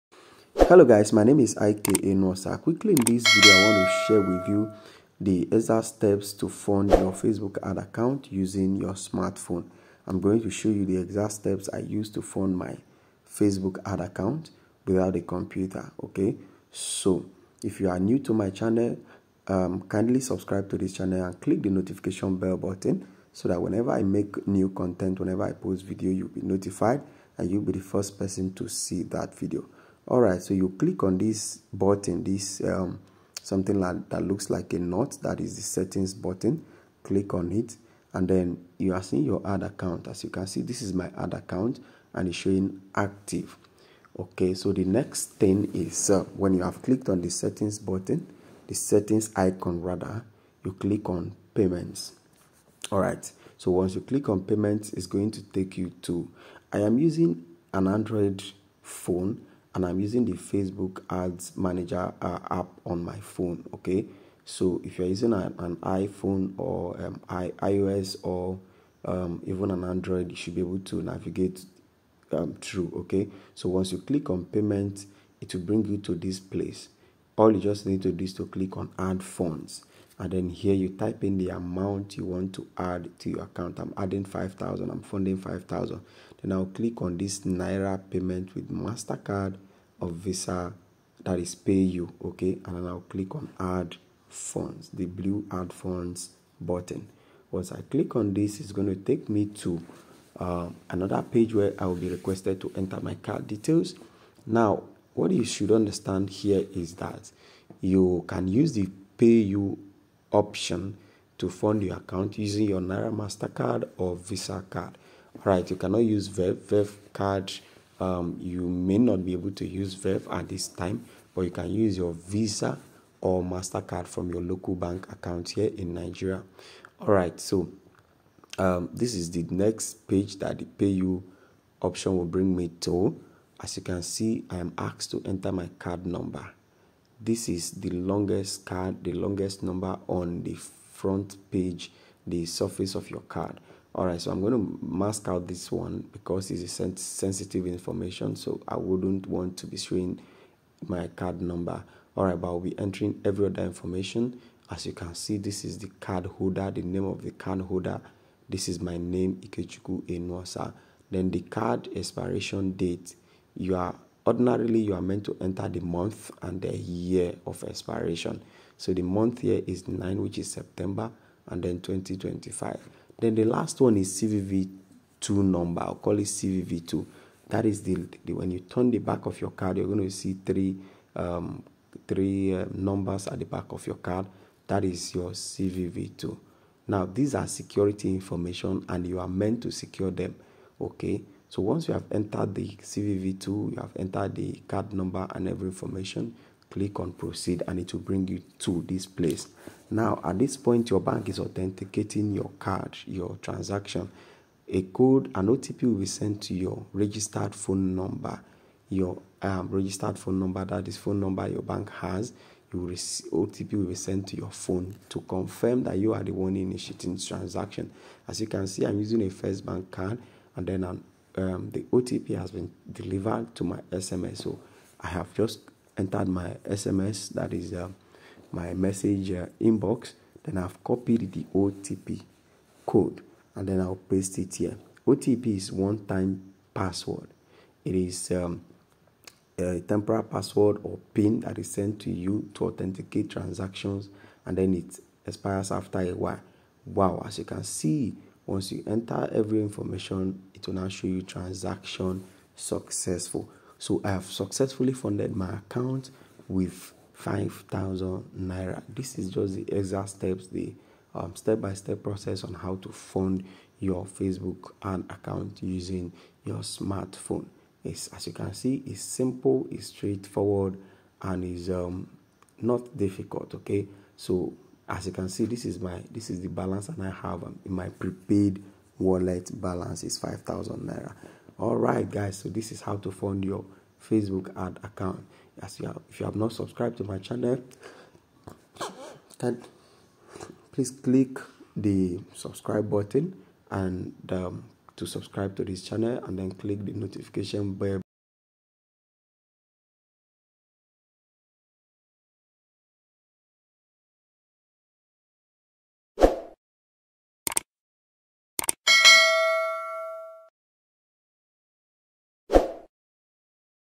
How to Pay for Facebook Ads in Naira | Meta Ads
A step by step guide to funding your Facebook Ad account in Nigeria using Naira Mastercard and Visa.

The payment gateway you will be using is called Payu.

Feel free to drop your comment, questions and concerns.